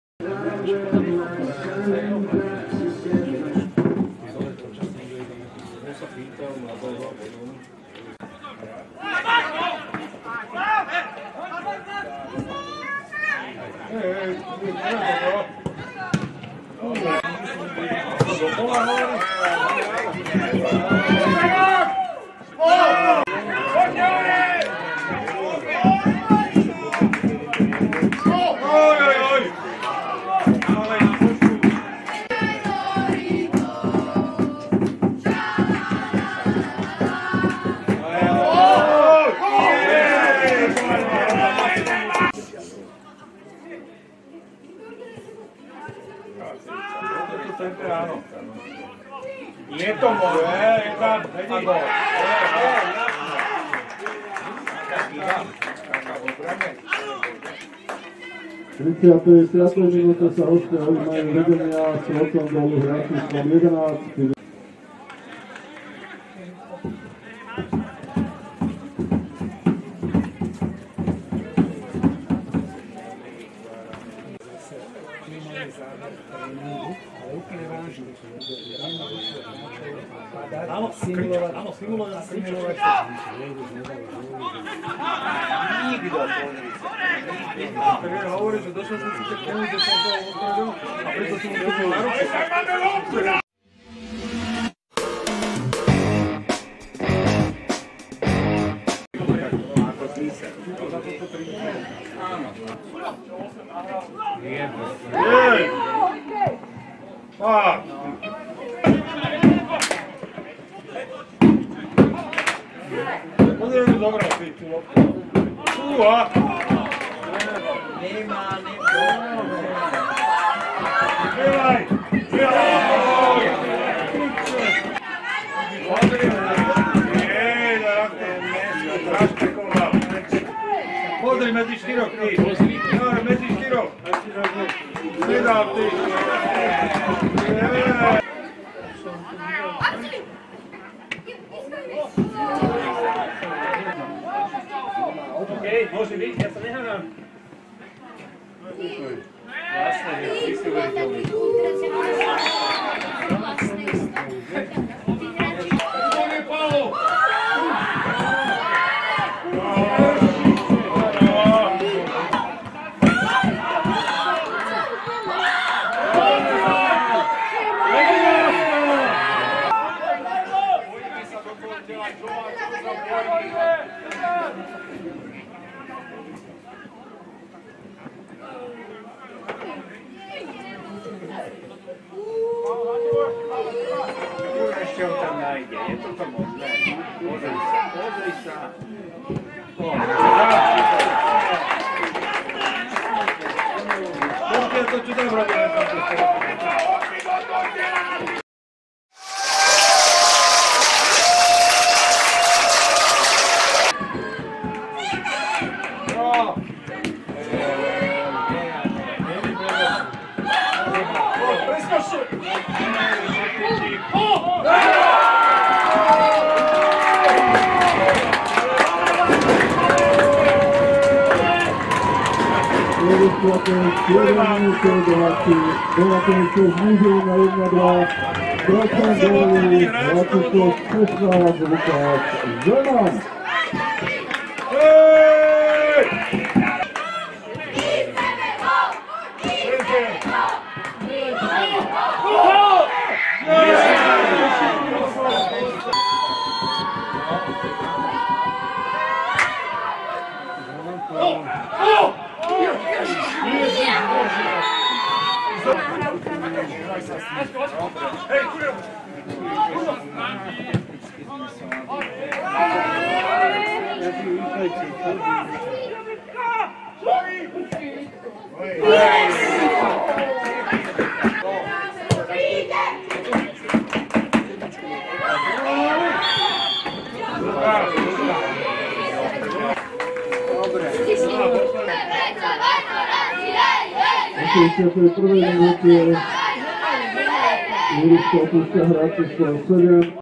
08 NAJ Vajnory-Volkswagen
Najepšie situácie
FK Vajnory – FC Volkswagen Bratislava 1-2 (0-1)
08.kolo, VI. liga muži S6A BFZ, ihrisko ŠK Vajnory, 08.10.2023
góly:
0-1 35´ Filip Juráň
0-2 65´ Paulo Augusto De Lima Gomes
1-2 77´ Lukáš Zeman